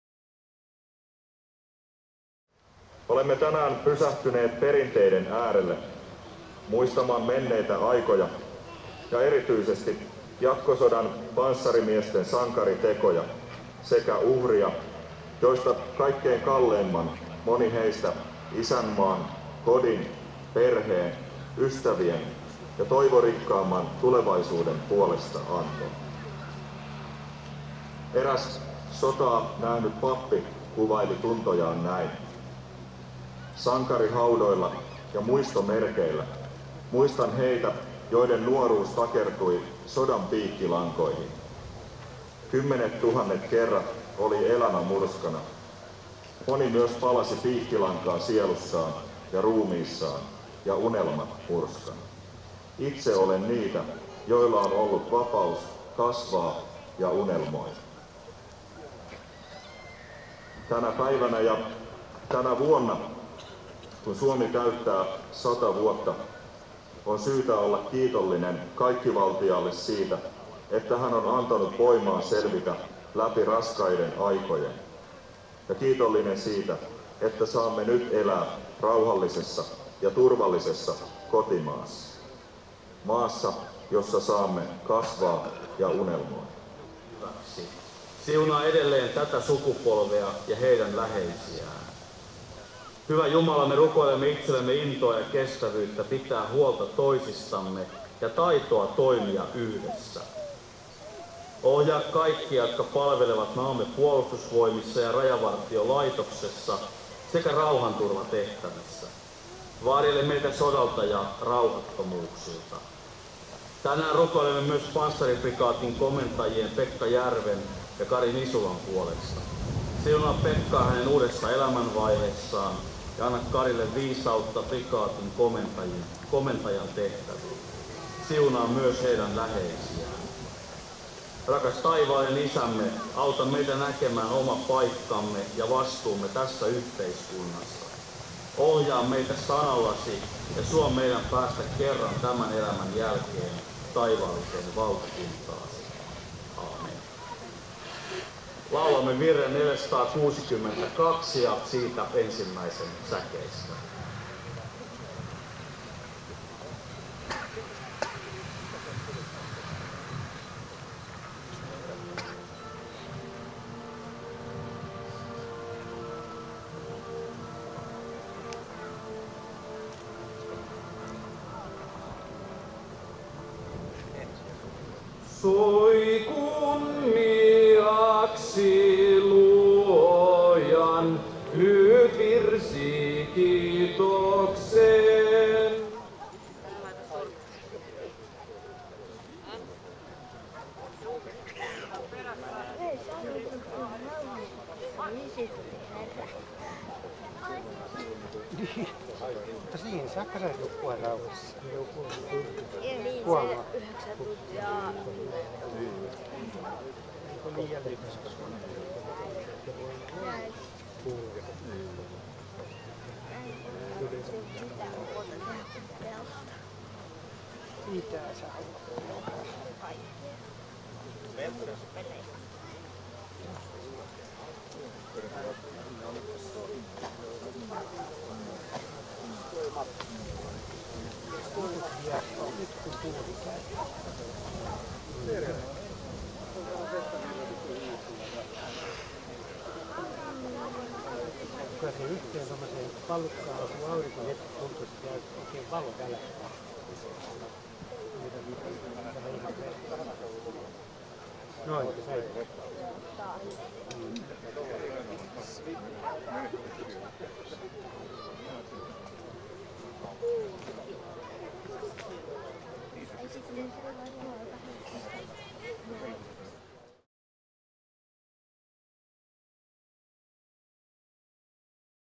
Finnish Armoured Division 75th Anniversary #4 - Sea of flags
Finnish Armoured Division 75  years anniversary - the flags to the starting point.
Panssaridivisioona 75 vuotta - liput siirtyvät lähtöpaikalle.

The Finnish Supreme headquarters ordered the foundation of an armoured division on 28 June 1942 and the actual foundation was on 30 June 1942. The division consisted of the newly formed Armoured Brigade and the old (1st) Jaeger Brigade. The Cavalry Brigade was also part of the division until January 1943. The division artillery consisted of the 14th Heavy Artillery Battalion. The division commander was Major General Ruben Lagus. During most of the war, the division was located at Petrozavodsk, but in the spring of 1944 it was moved to the Karelian Isthmus to form the reserve of the headquarters.
The Armoured Division fought in the crucial battles of 1944 and helped to contain the Soviet offensive.
The 75 years anniversary took place on 28.6. with parade, march past, inspection of troops and both modern and war time armored and motorized equipment parading past the audience and honored guests.

Panssaridivisioona 75 vuotta -tapahtuma on osa Hattulan kunnan ja Panssariprikaatin Suomi 100 -juhlallisuuksia Panssariprikaatissa Parolannummella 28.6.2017. Tapahtumassa on mukana myös kokonaisturvallisuuden juhlahanke NUKU RAUHASSA.
Panssariprikaati juhlii perinteisesti vuosipäiväänsä 28.6. Kyseisenä päivänä  Äänislinnassa vuonna 1942 perustettiin Panssaridivisioona, jolla oli ratkaiseva rooli kesän 1944 torjuntataistelussa nykyaikaisimpana ja iskukykyisimpänä joukkonamme. Tästä joukosta sai alkunsa nykyinen Panssariprikaati.